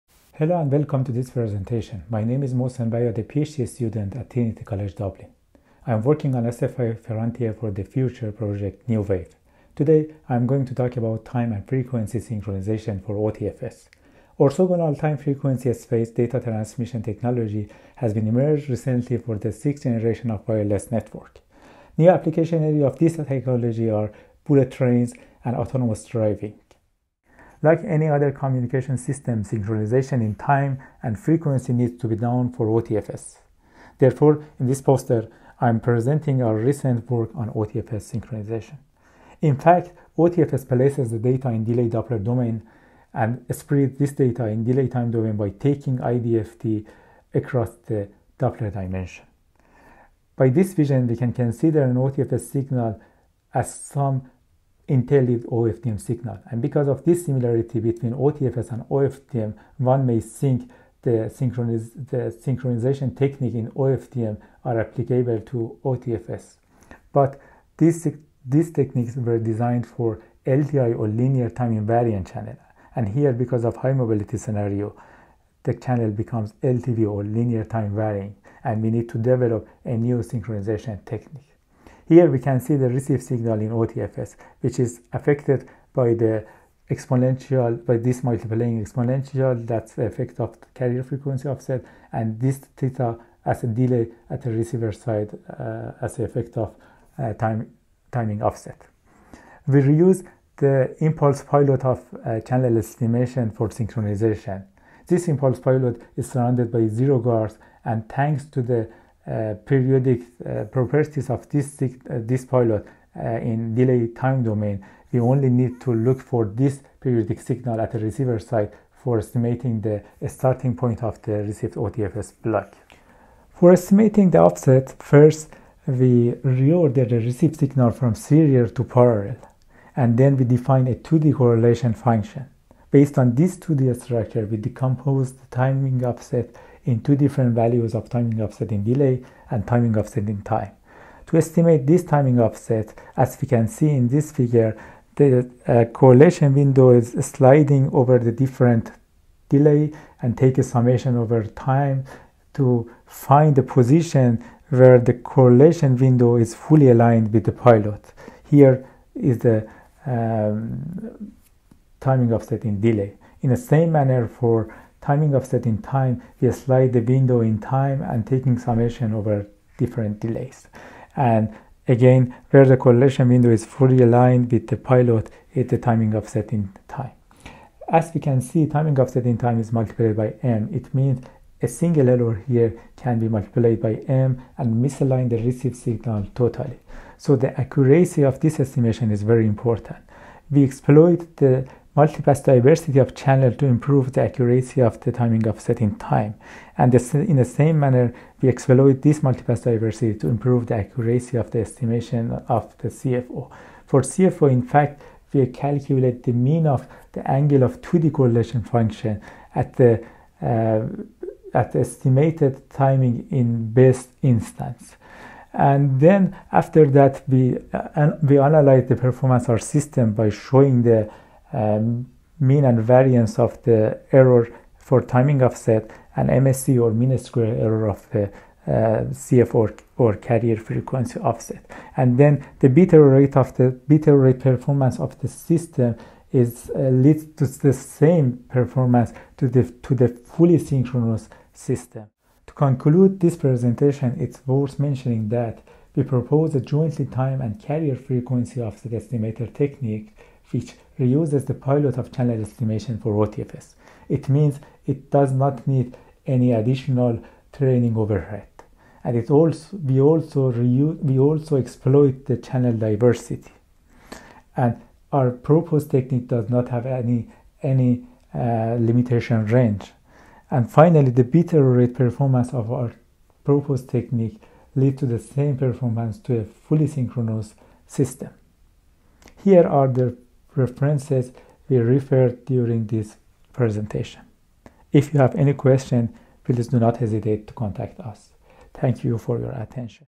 SFI Summit 2022: Moshen Bayat(TCD)'Time and Frequency Synchronization for OTFS'
PhD student Moshen Bayat presents research poster on 'Time and Frequency Synchronization for OTFS'.

In this poster, we propose timing offset (TO) and carrier frequency offset (CFO) estimators for orthogonal time frequency space modulation (OTFS). The proposed estimators do not require any additional training overheads as they deploy the same pilot signal that is used for channel estimation. Hence, no additional training overhead is required for synchronization, thanks to the periodic properties of the pilot signal in the delay-time domain.

Our proposed TO estimator finds the start of each OTFS block by searching for a periodic sequence in delay and time dimensions. The CFO is then estimated by finding the mean of the angles of a two-dimensional correlation function at the best timing instant.

As a novel aspect of these estimators, the multipath diversity of the channel is exploited to achieve a high estimation accuracy. Finally, we show the efficacy of our proposed synchronization techniques through extensive simulations.